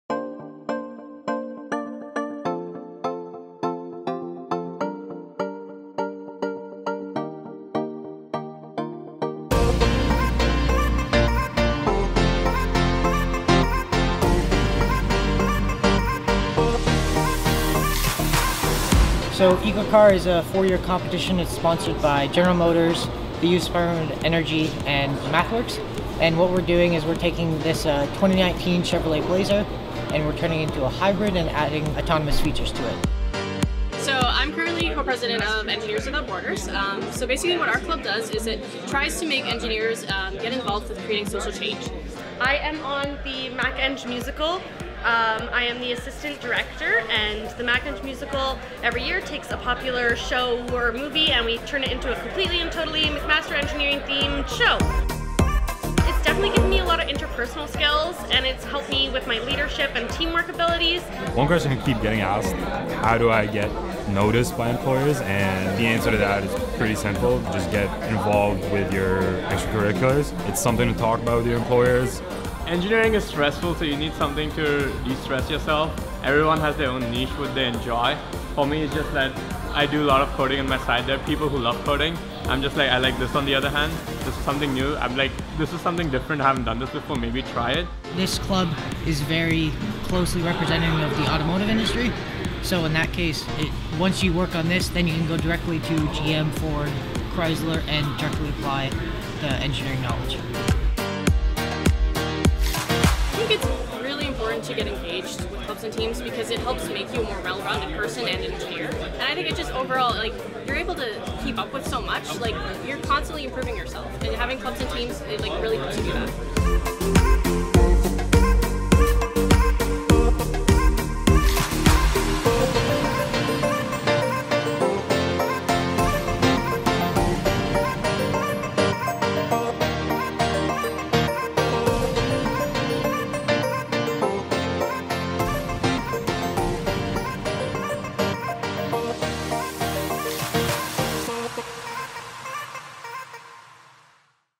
Explore Clubs and Teams | McMaster Engineering
Learn from McMaster University engineering students engaged in the community and explore the wide variety of clubs and teams offered with McMaster Engineering. From Baja racing to Women in Engineering outreach, three student ambassadors speed-tour the clubs and design teams that turn theory into leadership skills. See how joining just one group can add soft-skill points that recruiters notice.

Learn more about Canadian Engineering Programs and student life at McMaster